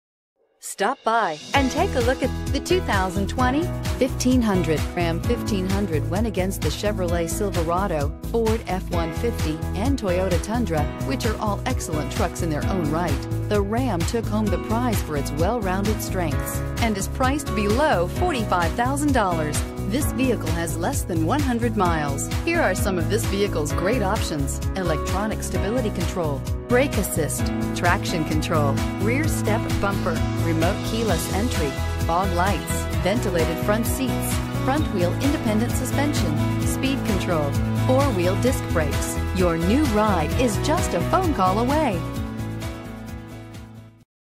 2020 RAM 1500 Forest Lake MN R20182
2020 RAM 1500 Laramie

2020 Ram 1500 Laramie Priced below KBB Fair Purchase Price! 4D Crew Cab Bright White Clearcoat HEMI 5.7L V8 Multi Displacement VVT eTorque 4WD 8-Speed Automatic LIFETIME POWERTRAIN WARRANTY INCLUDED!* $1,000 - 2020 National Retail Consumer Cash 20CL1. Exp. 12/02/2019, $1,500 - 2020 Retail Bonus Cash 20CLA1. Exp. 12/02/2019